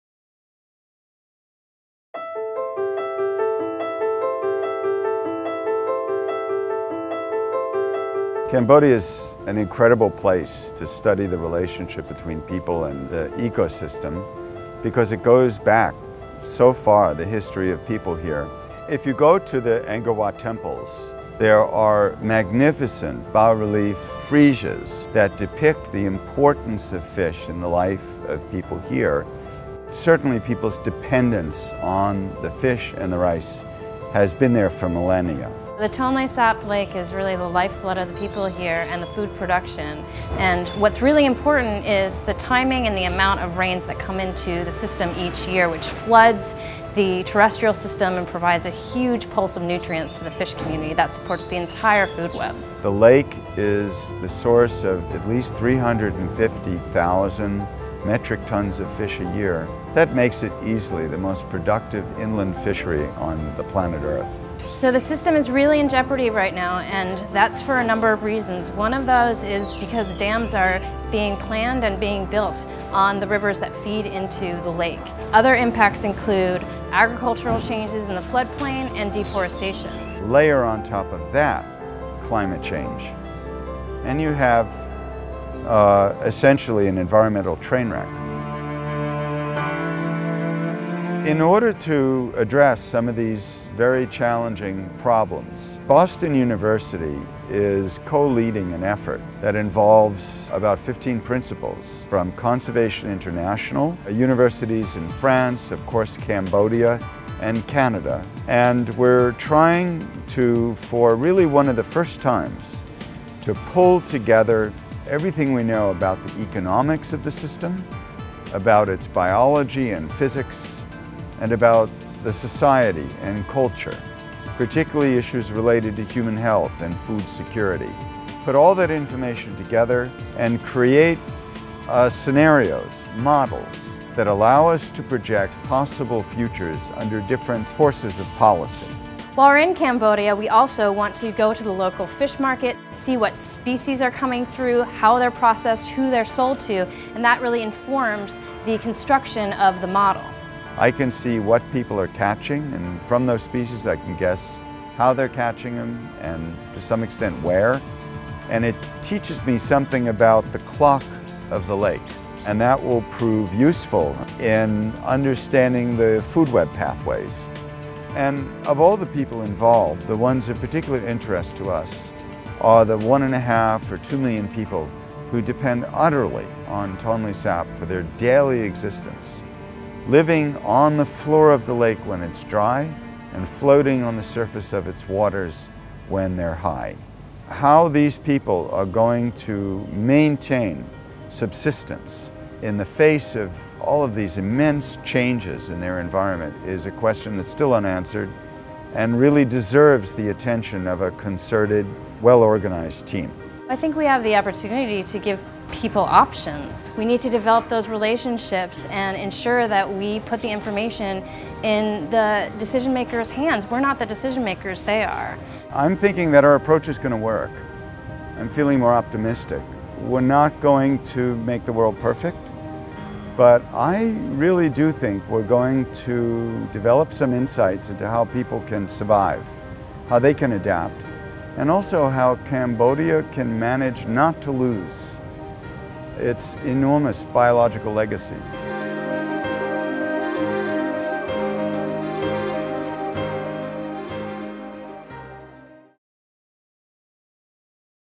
Trouble at Tonle Sap Lake
Professor Les Kaufman and a team of researchers from Convservation International and universities in the United States, France, and Canada, have gathered in Cambodia to examine and model the food webs at Tonle Sap Lake, the country's primary source for fish, rice, and protein.  With this data, they hope to present solutions that will allow the lake's pulse system to continue as it has for centuries, in spite of the effects of dam-building, modernization, and climate change.